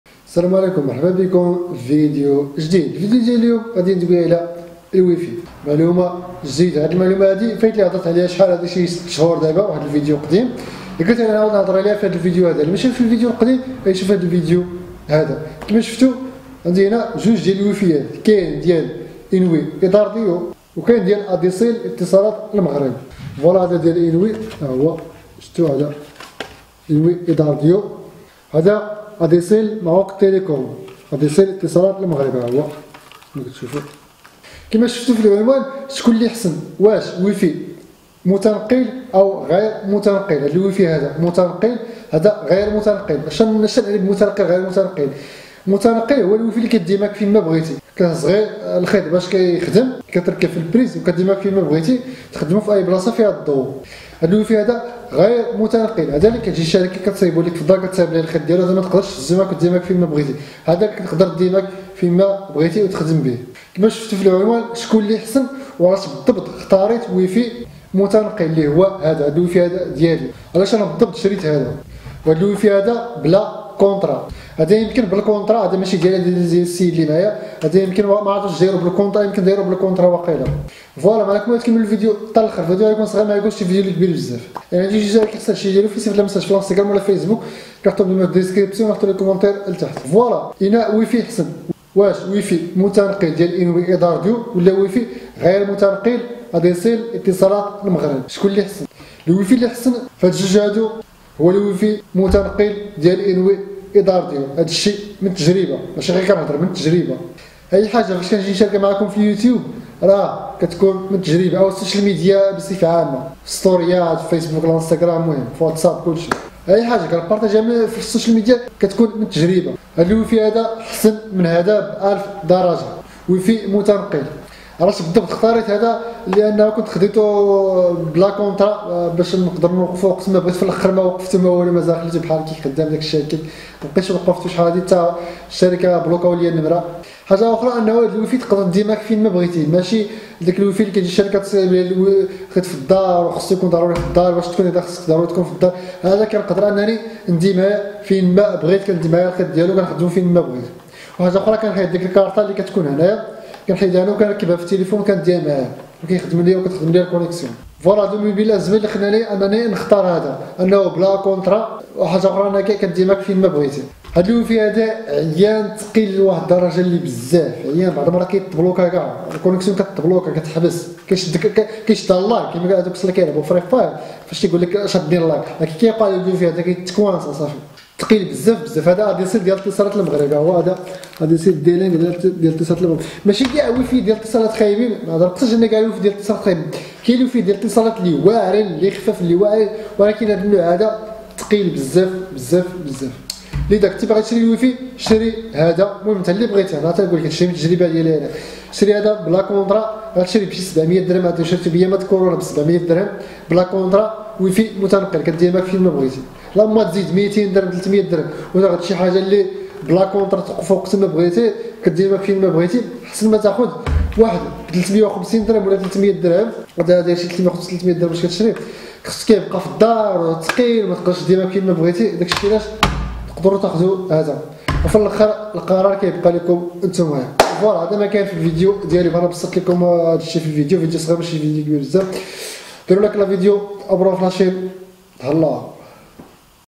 من الأفضل WiFi inwi idar duo 🆚 Adsl Maroc Telecom ويفي متنقل ويفي غير متنقل - لماذا إخترت هذا الويفي
هذه القناة تعمل على توفير فلوغات وإستكشاف أماكن جديدة